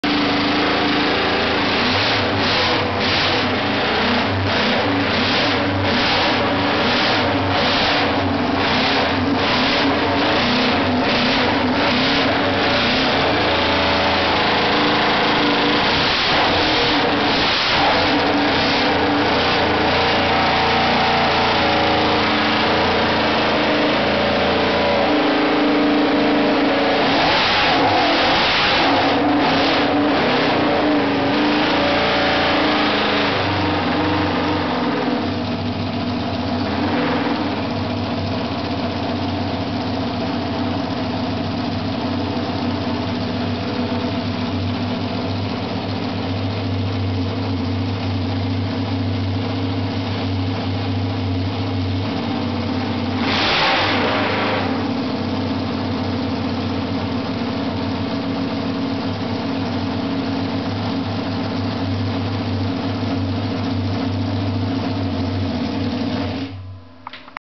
Suzuki Straight 500 8 homemade motorcycle New 2011 Video
Suzuki 500/8 2011 spec using modified carbs (Amal375)s and new Exhausts .
Sounds awsome and very loud